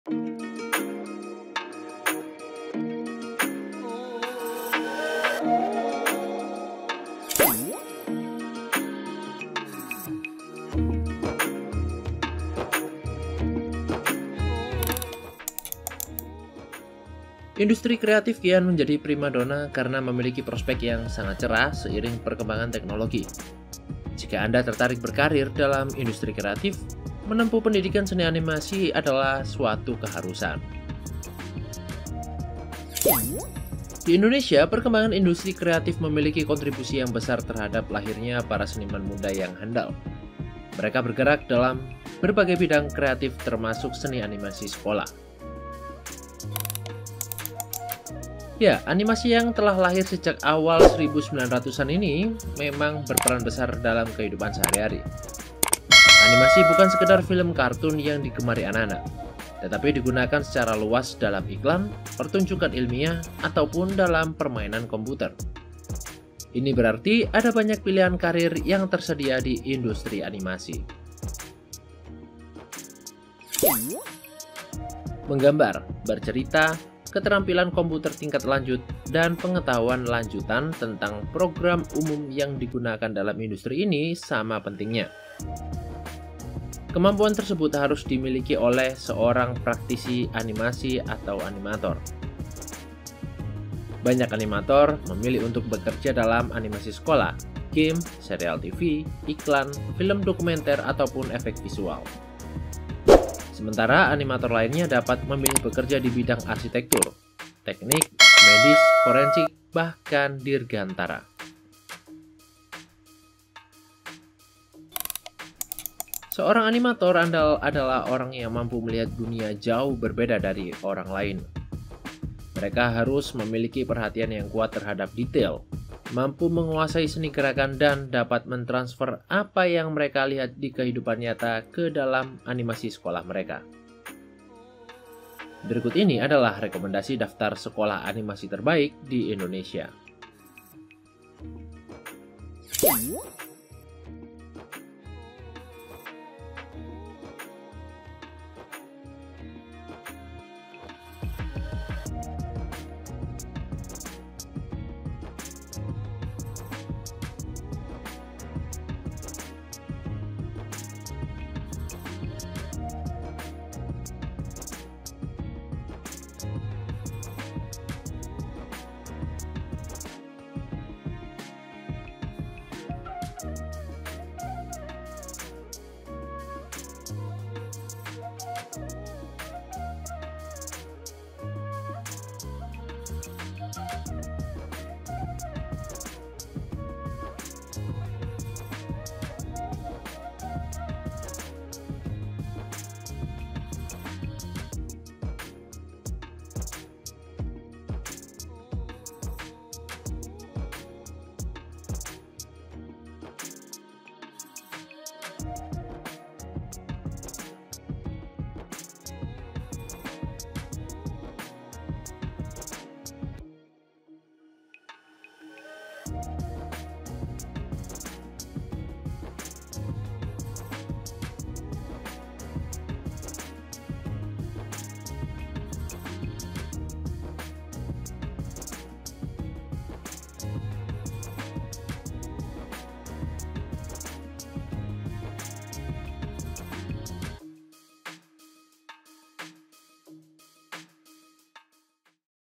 Rekomendasi 5 Sekolah Animasi Terbaik Untuk Anak di Indonesia
Industri kreatif kian menjadi primadona karena memiliki prospek yang sangat cerah seiring perkembangan teknologi. Jika Anda tertarik berkarier dalam industri kreatif, menempuh pendidikan seni animasi adalah suatu keharusan.

Di Indonesia, perkembangan industri kreatif memiliki kontribusi yang besar terhadap lahirnya para seniman muda yang andal. Mereka bergerak dalam berbagai bidang kreatif, termasuk seni animasi sekolah.